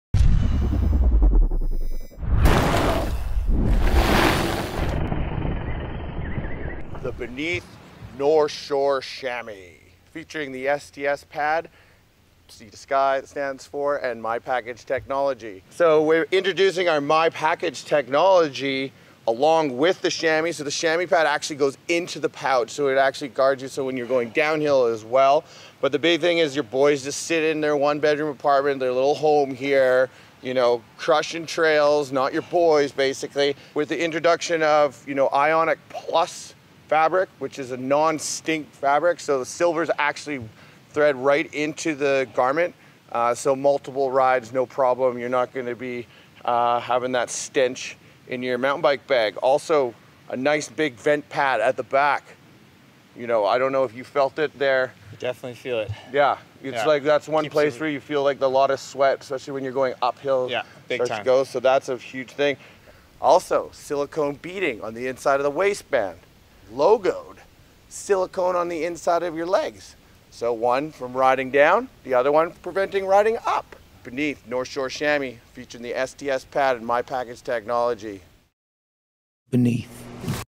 4. BN3TH - North Shore Chamois - Product Knowledge
Watch the training video for the BN3TH North Shore Chamoisf. After you have watched the video answer some questions

First of its kind, the new North Shore Chamois, featuring BN3TH's patent-pending Sea-to-Sky (STS) Pad™ combined with their supportive MyPakage Pouch Technology™, create a new world of comfort designed for the ultimate ride. Plus, the use of permanent silver anti-odor iconic+™ technology kills stink before it starts, making the ride up as much fun as the ride down.